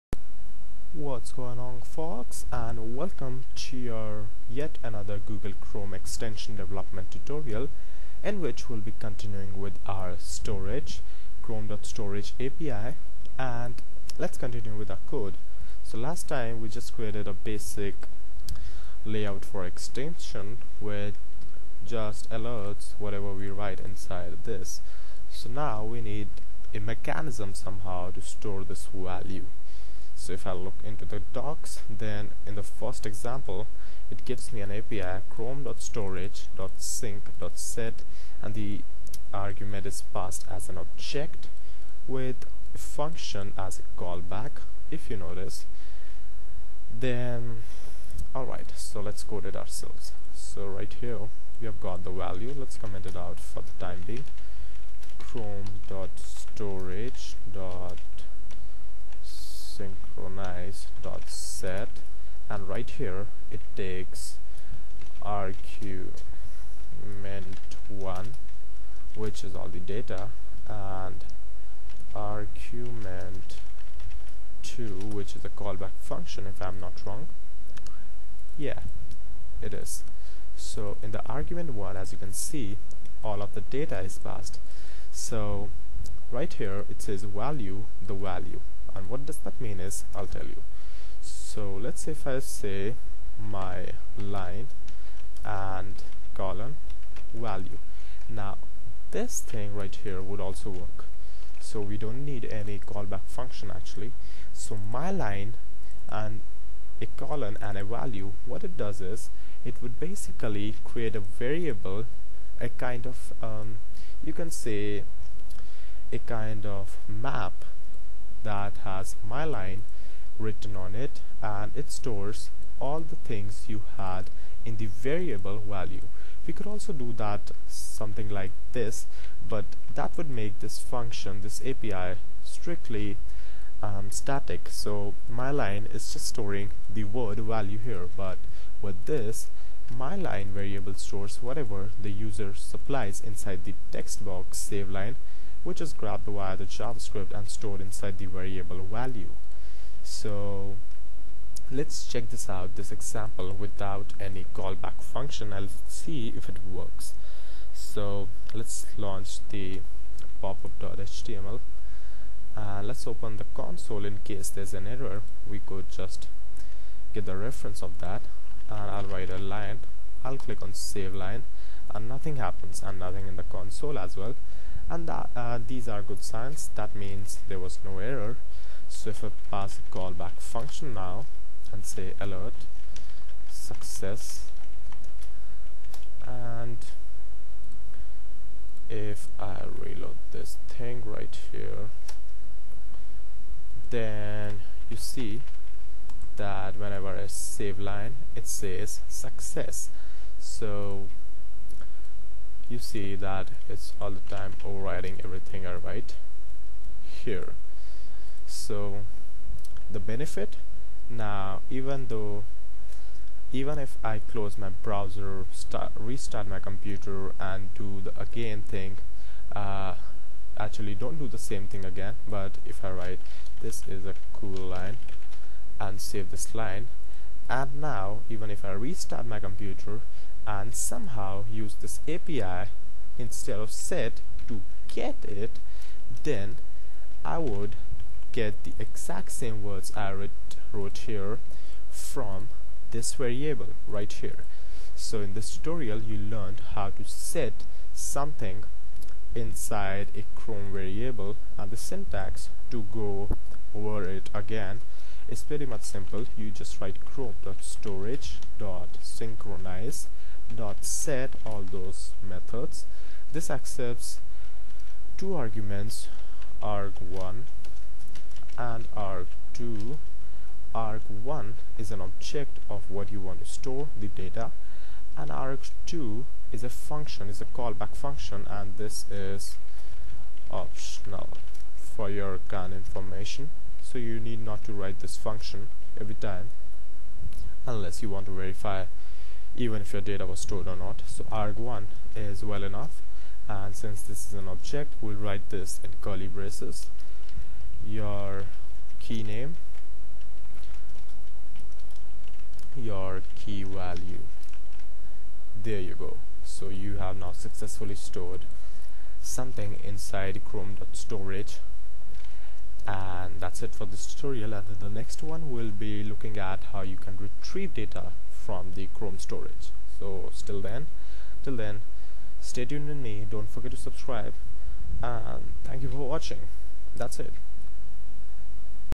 Chrome Extension Tutorial 25: Developing Extension On chrome.storage - Part 1
Learn how to develop extensions making use of chrome.storage API to store user data!